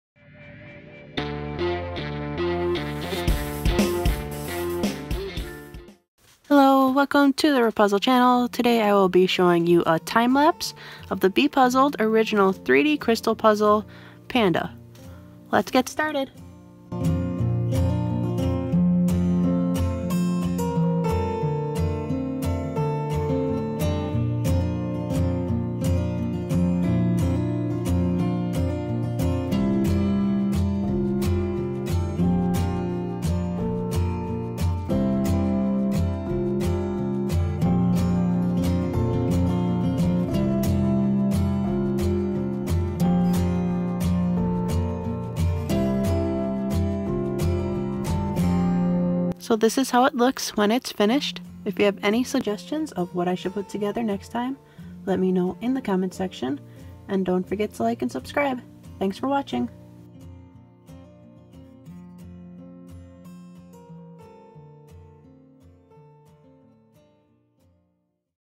Bepuzzled 3D Crystal Puzzle- Panda- Time Lapse Version
This video is a time lapse of the bepuzzled original 3d crystal puzzle panda. This video is not meant to be used as a tutorial but you can click on the card at the start of the video to get to the panda and baby tutorial and you'll be able to use that to get this puzzle together. This is a level 1 puzzle with 41 pieces.